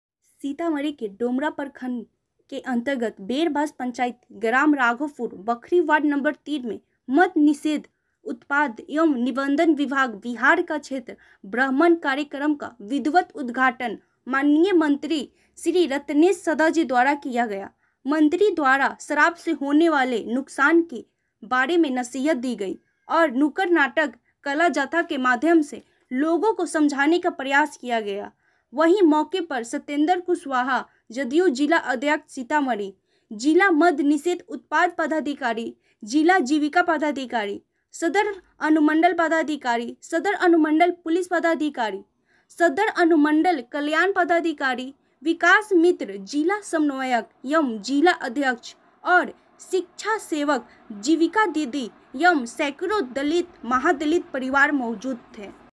सीतामढ़ी में मध-निषेध,उत्पाद एवं निबंधन विभाग बिहार का क्षेत्र भ्रमण कार्यक्रम का विधिवत उद्घाटन।।
सीतामढी के डुमरा प्रखंड अंतर्गत बेरबास पंचायत-ग्राम राघोपुर बखरी वार्ड न0-03 मे मध-निषेध,उत्पाद एवं निबंधन विभाग बिहार का क्षेत्र भ्रमण कार्यक्रम का विधिवत उद्घाटन माननीय मंत्री श्री रत्नेश सदा जी   द्वारा किया गया।

मंत्री  द्वारा शराब से होनेवाले नुकसान के बारे मे नसीहत दी गई,

और नुक्कड नाटक/ काला जत्था, के माध्यम से लोगो को समझाने का प्रयास किया गया।

वही मौक पर सत्येंद्र कुशवाहा जद यू जिलाध्यक्ष सीतामढी,

जिला मध-निषेध उत्पाद पदाधिकारी,
जिला जीविका पदाधिकारी ,
सदर अनुमंडल पदाधिकारी,
सदर अनुमंडल पुलिस पदाधिकारी,
सदर अनुमंडल कल्याण पदाधिकारी,
विकास मित्र जिला समन्वयक एवं  जिलाध्यक्ष जिलाध्यक्ष
 और विकास मित्र/शिक्षा सेवक/जीविका दीदी एवं सैकड़ो दलित-महादलित परिवार  मौजूद थे।